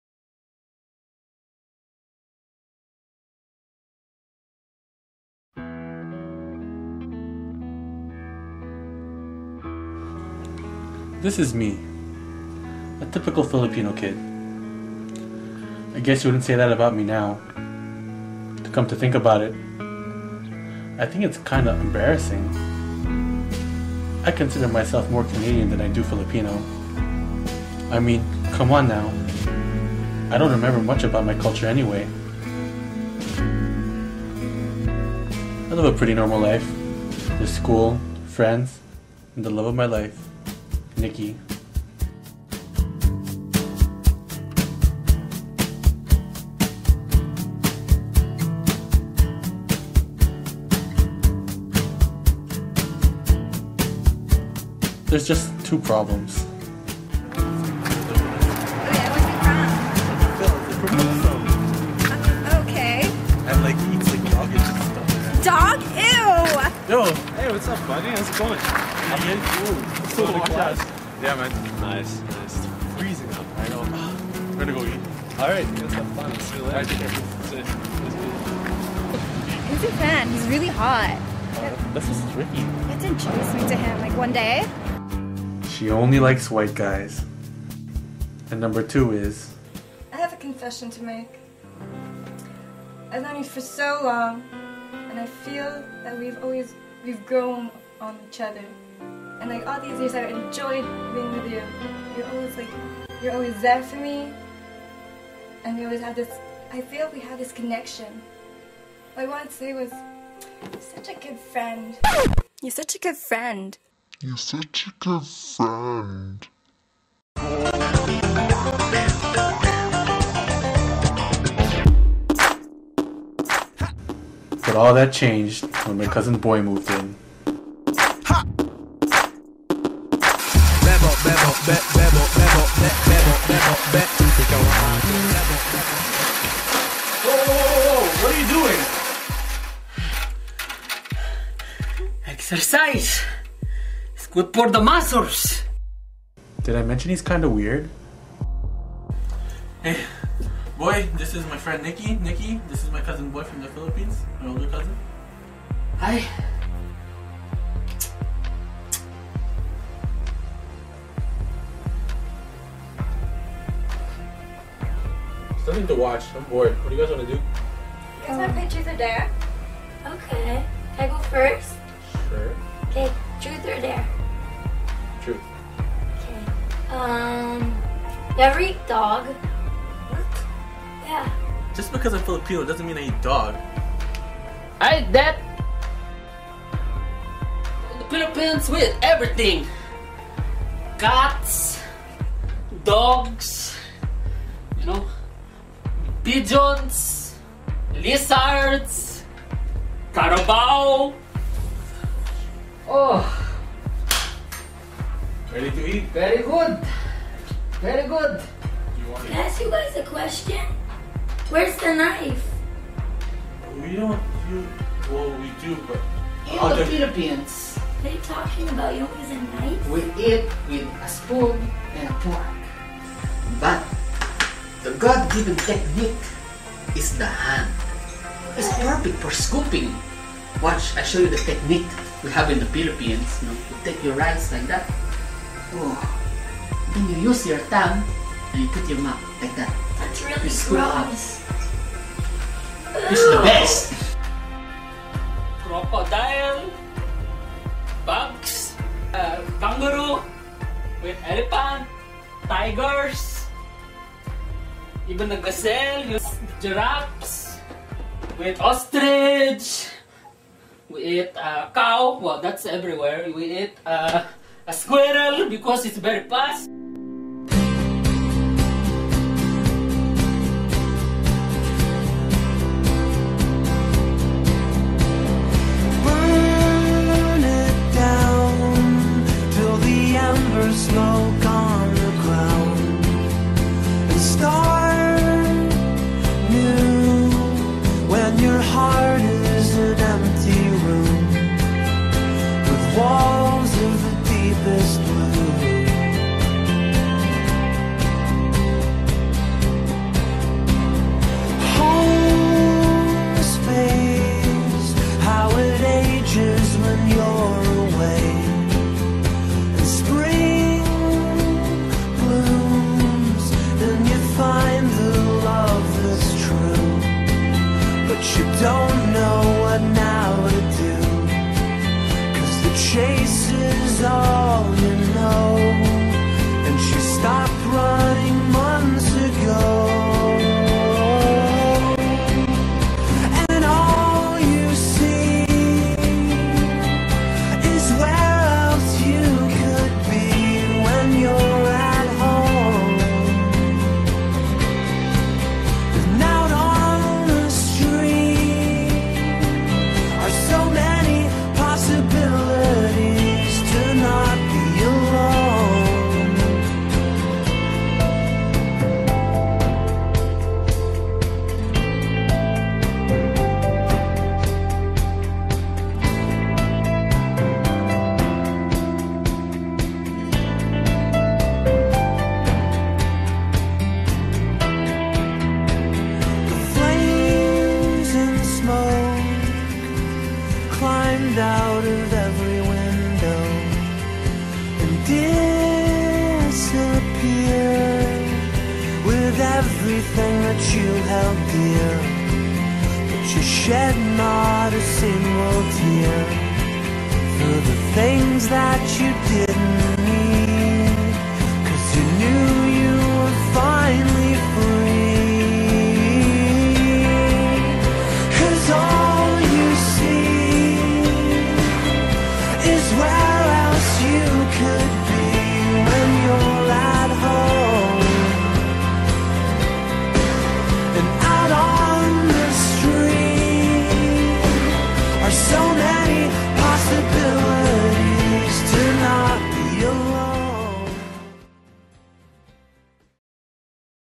BOY
A film made for our Theology Class. Edited by and starring Phil Penalosa. Also Starring J.E. Jalea, Erin Talbot, and Nikki Coronado